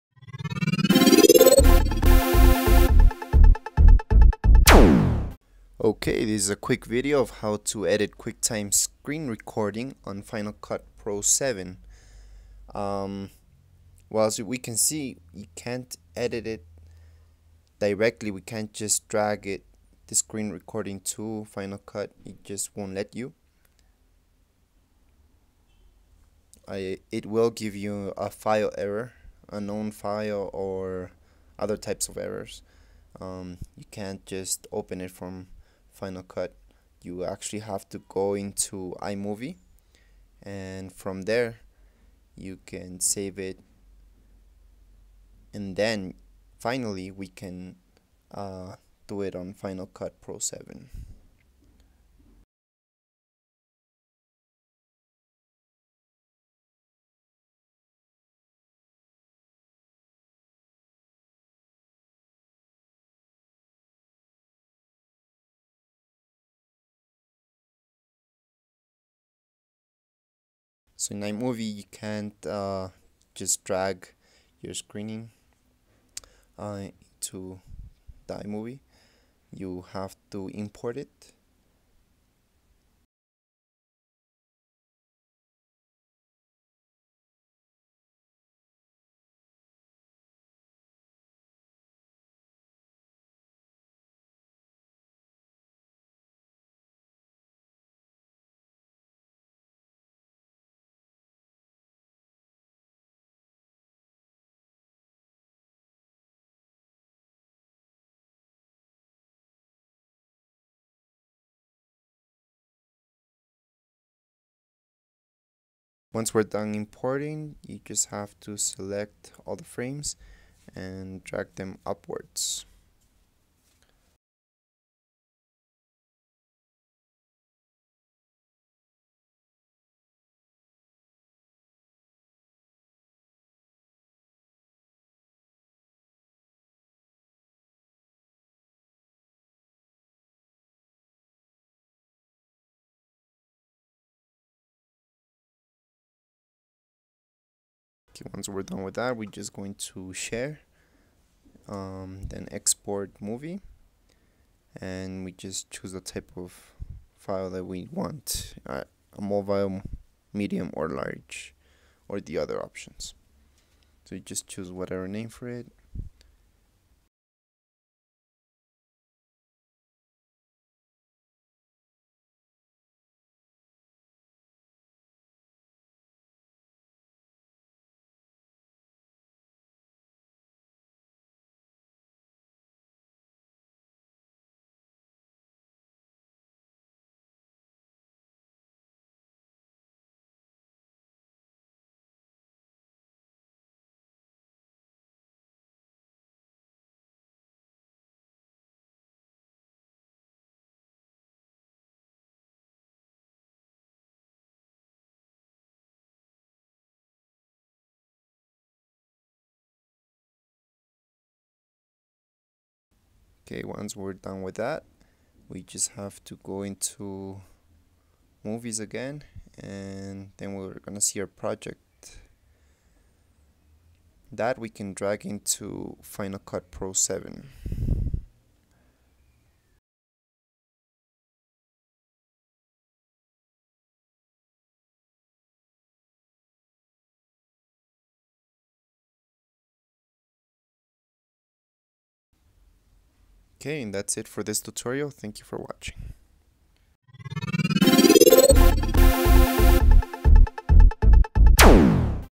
how to edit quicktime screen recording in Final Cut Pro 7
video of how to edit Quicktime screen recording in Final Cut pro 7 using iMovie.